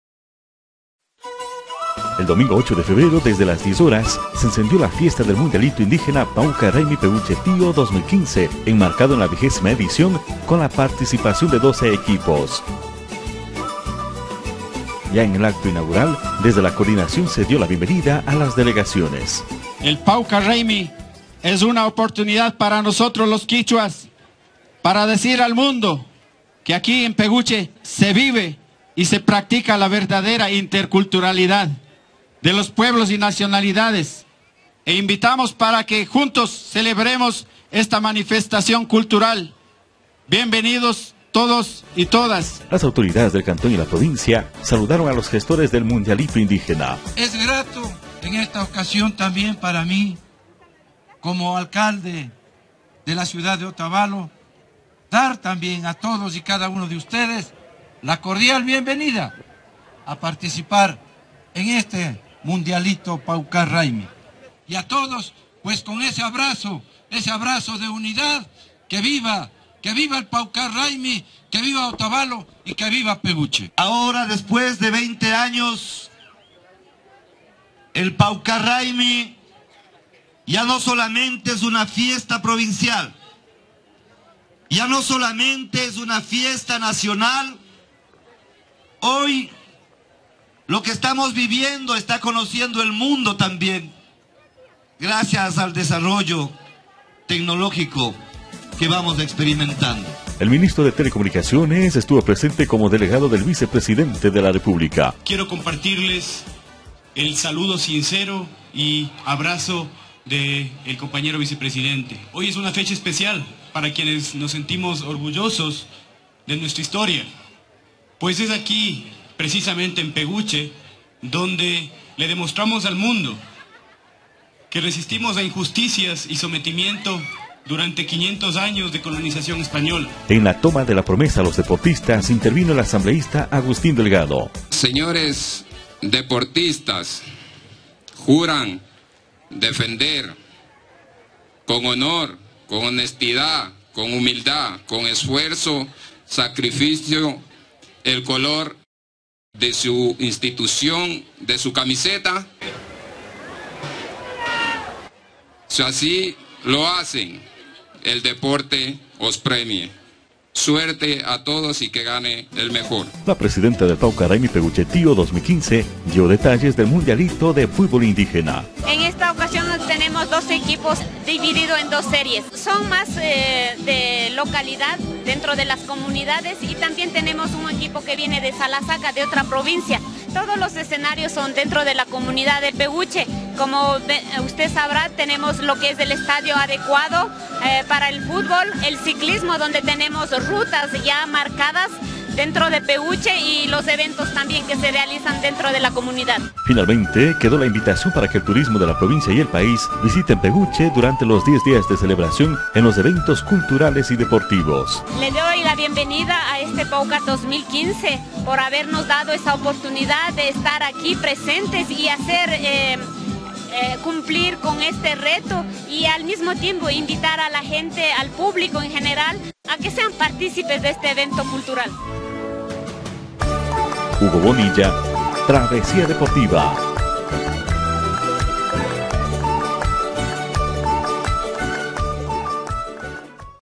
Inició la fiesta del Pawcar Raymi Peguche Tío (Noticias Ecuador)
El domingo 8 de febrero desde las 10h00 se activó el mundialito de fútbol indígena Pawkar Raymi Peguche Tío 2015, enmarcado en la vigésima edición con la participación de 12 equipos.